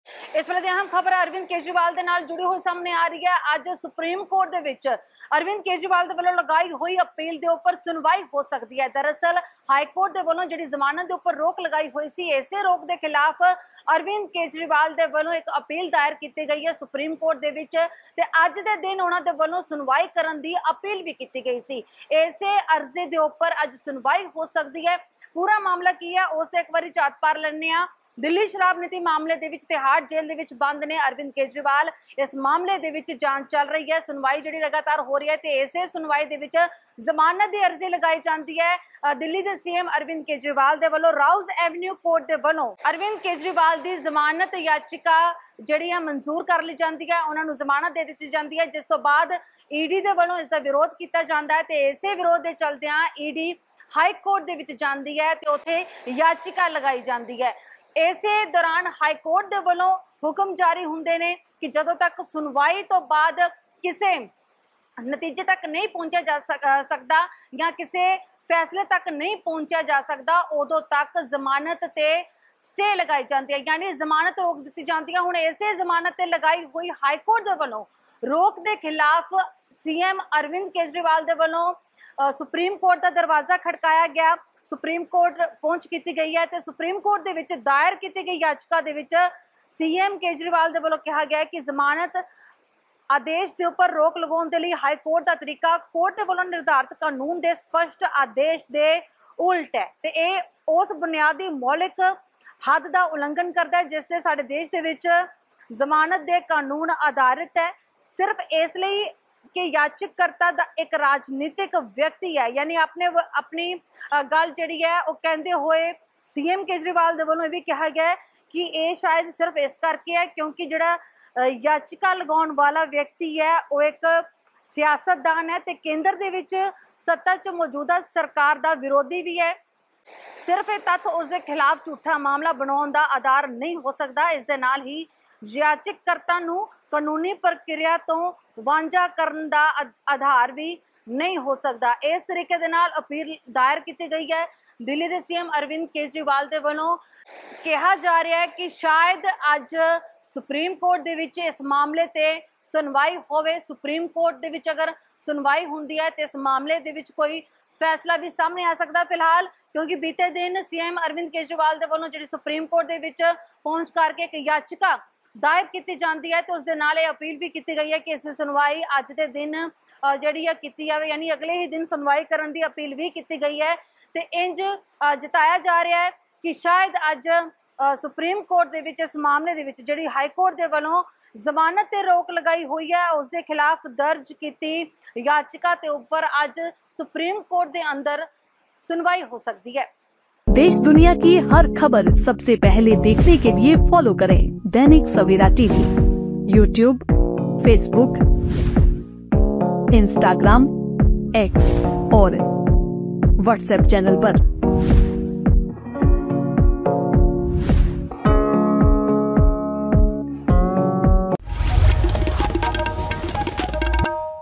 Arvind Kejriwal ਦੀ ਜ਼ਮਾਨਤ ਸਟੇਅ ਮਾਮਲੇ 'ਤੇ ਅੱਜ ਸੁਪਰੀਮ ਕੋਰਟ 'ਚ ਹੋ ਸਕਦੀ ਹੈ ਸੁਣਵਾਈ
Arvind Kejriwal's bail stay case may be heard in the Supreme Court today